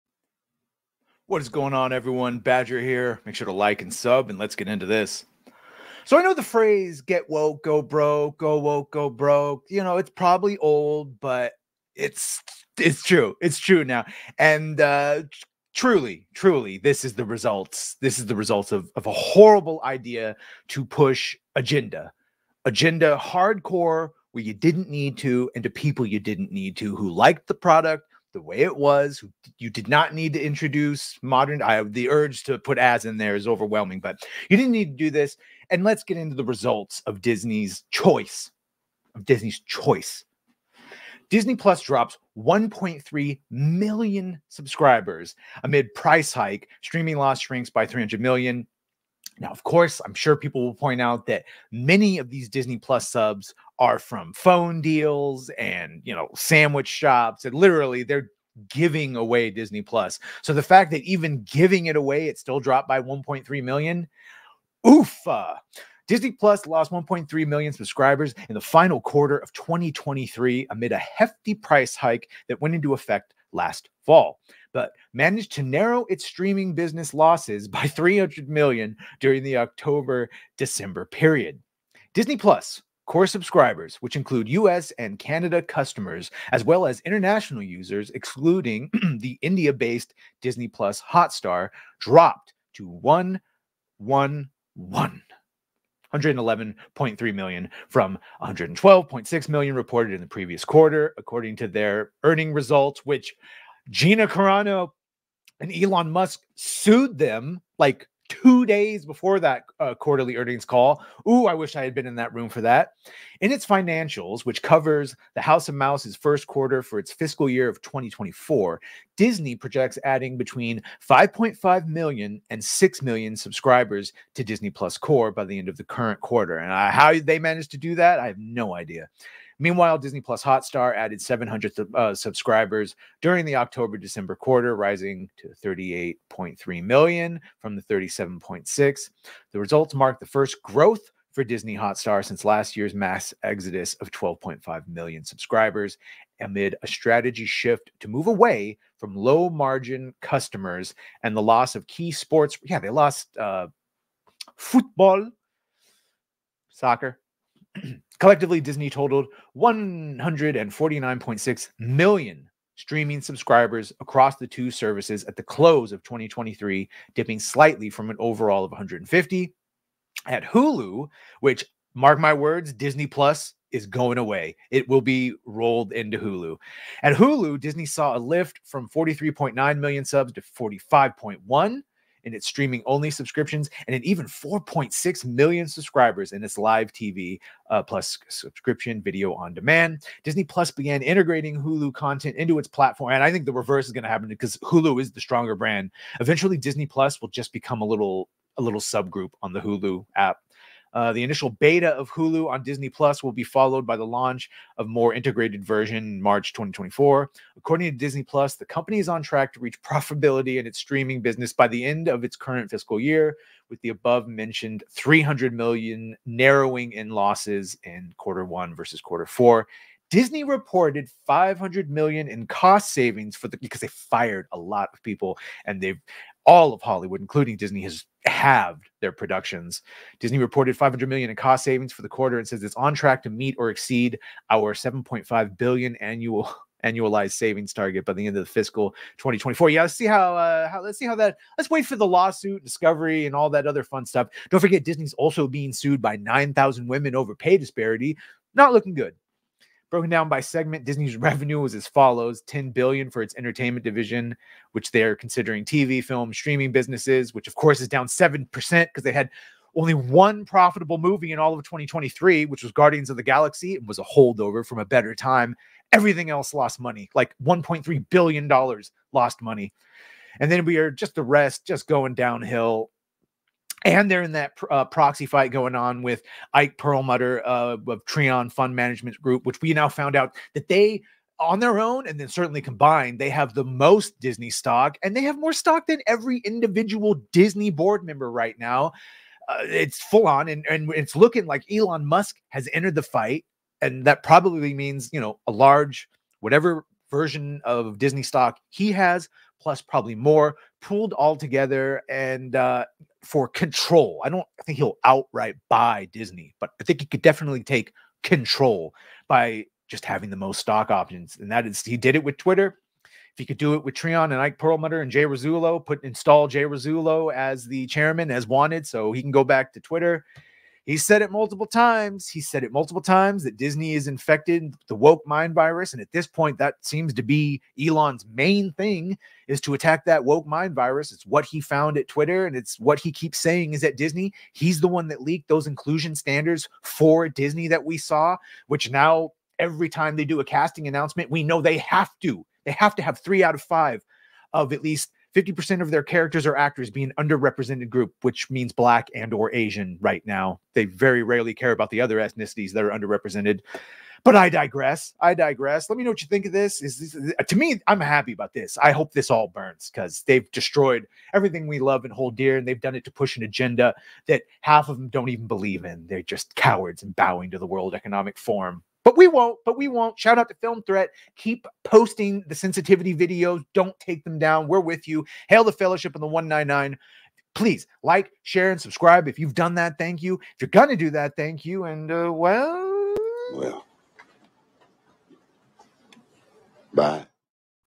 Disney Plus Loses 1.3 Million Subscribers! 300 Million Gap!
Badger talks about the huge drops in Subs for Disney Plus.